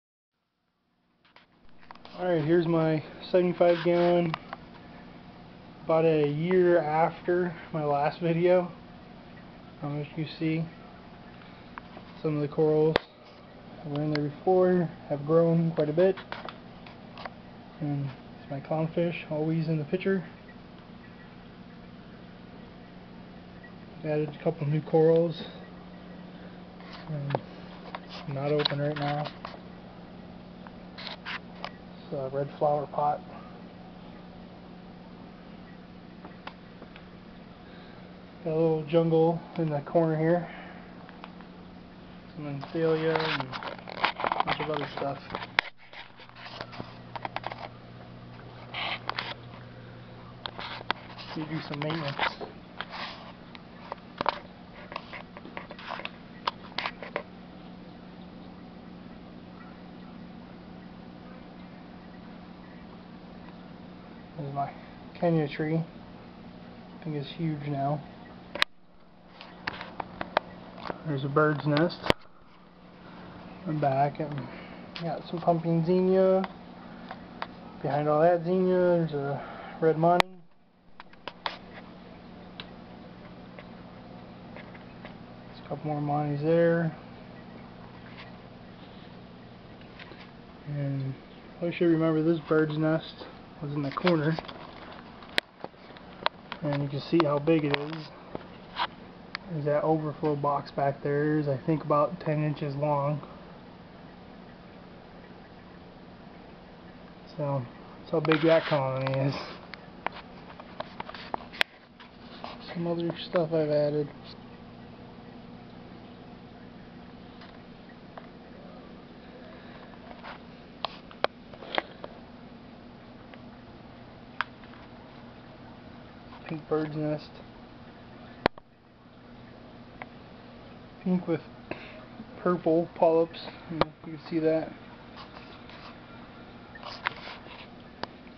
75 gallon reef tank Part 2
about a year after me first video. sorry it took so long to post,  My 75 gallon reef tank is doing great.  Since adding the calcium reactor my coral growth is fantastic. I will never set up a tank without 1 again. let me know what u think.